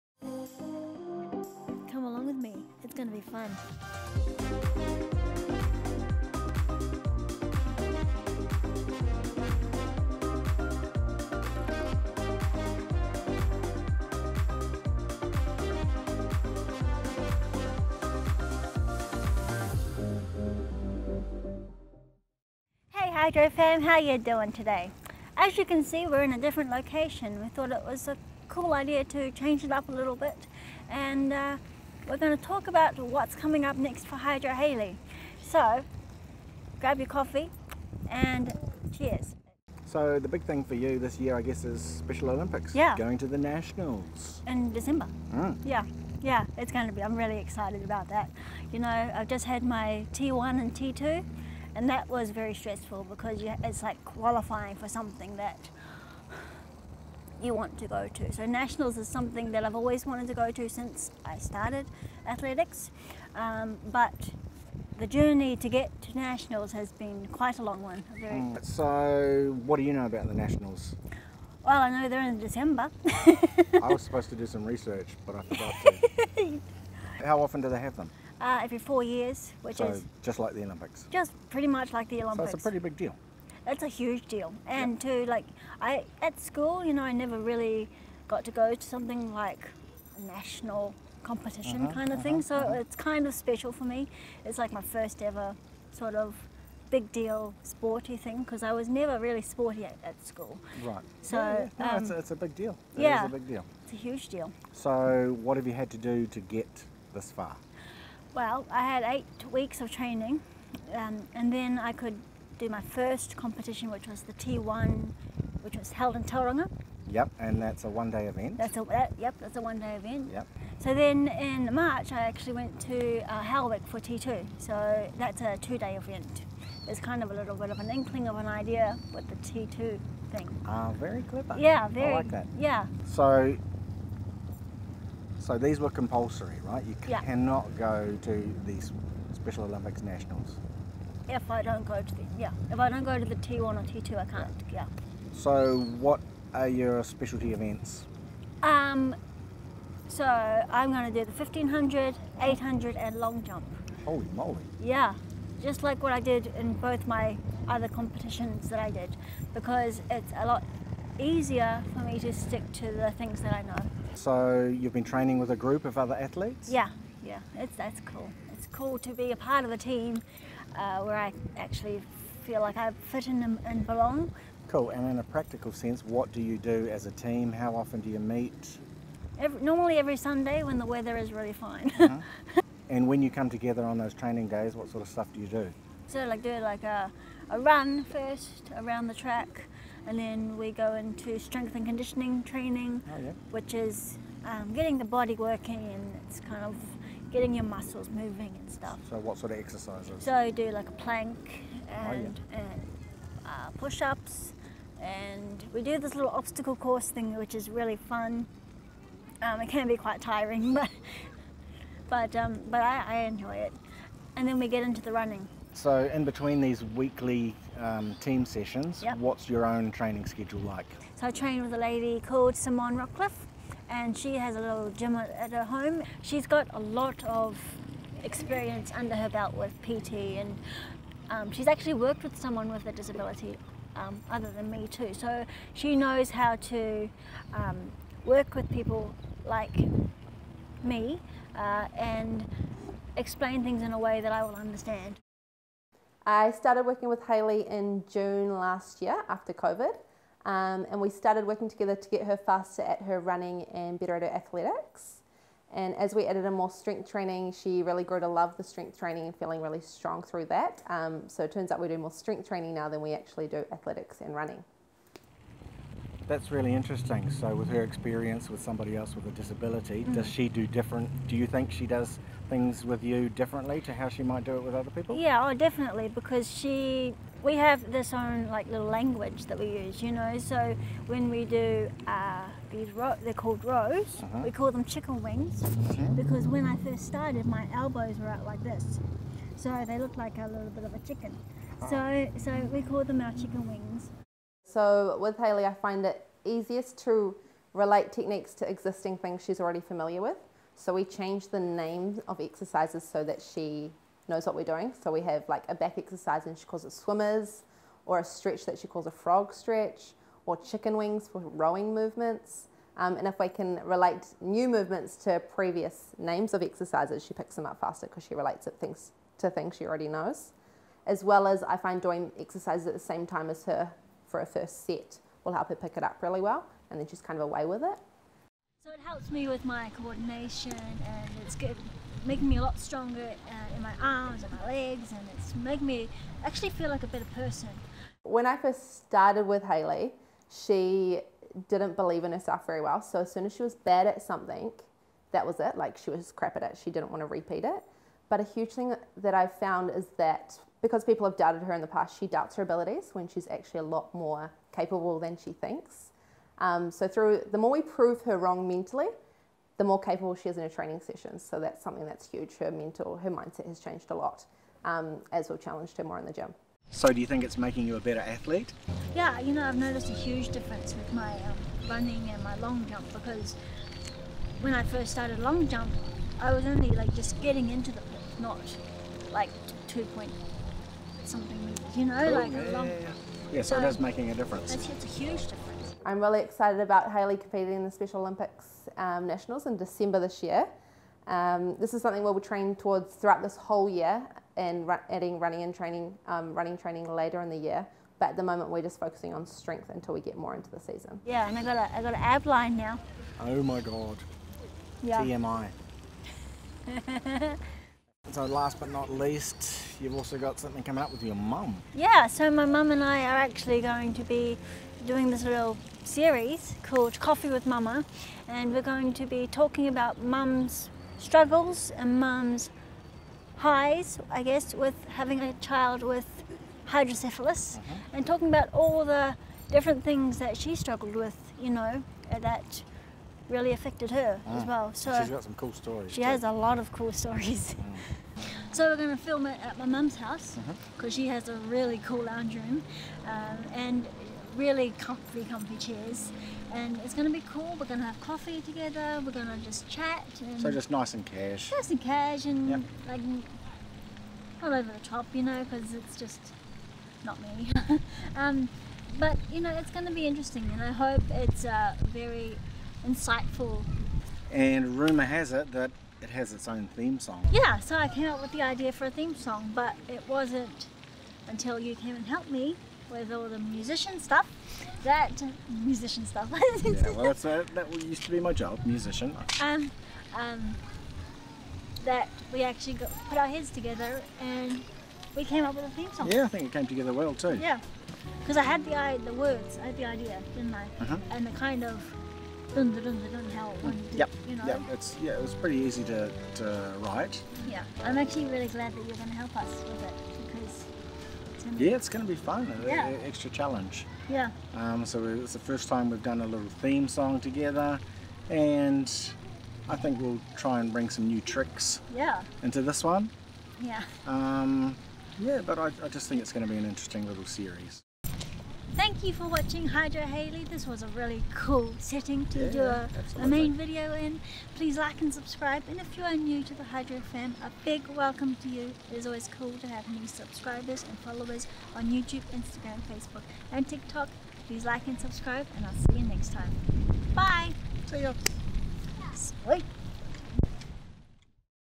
Hydro Hayley update 2021 l Hydro Hayley
Hey Hydro fam - in this video i talk with my friend Derrin about all the things that I have been doing and are going to be doing for the rest of the year. I love changing up the scenery too. Nice to be outside! Hope you enjoy this video!

"Every day's a challenge, so let's embrace it!"